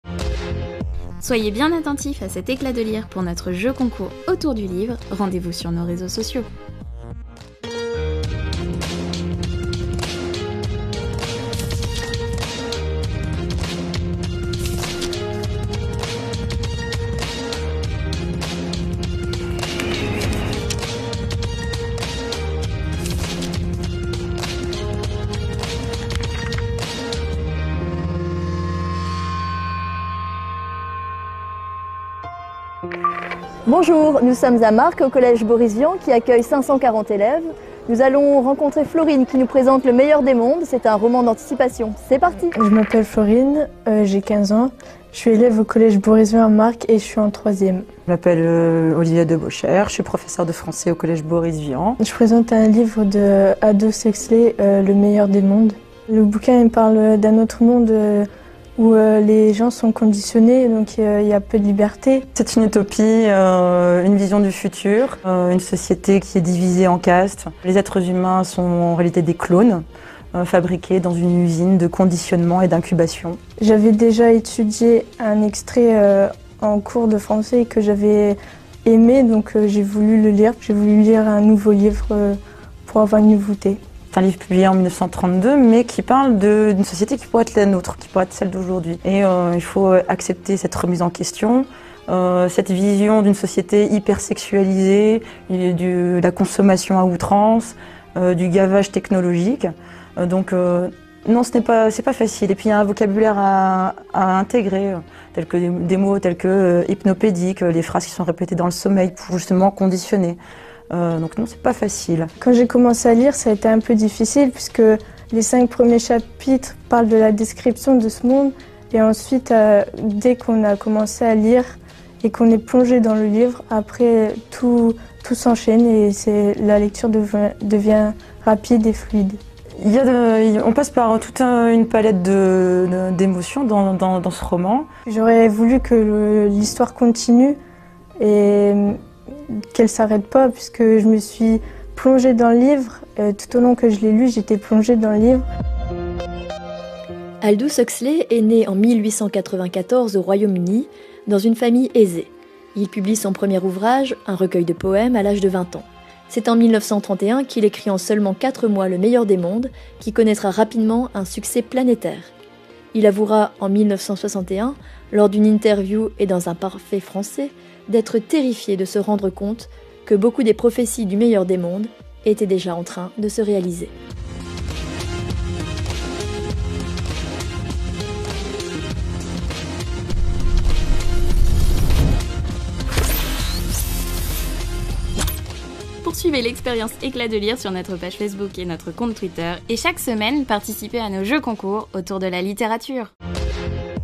Eclat de Lire: le meilleur des mondes d'Aldous Huxley
"Eclat de Lire"  s'intéresse aujourd'hui au livre "le meilleur des mondes" d'Aldous Huxley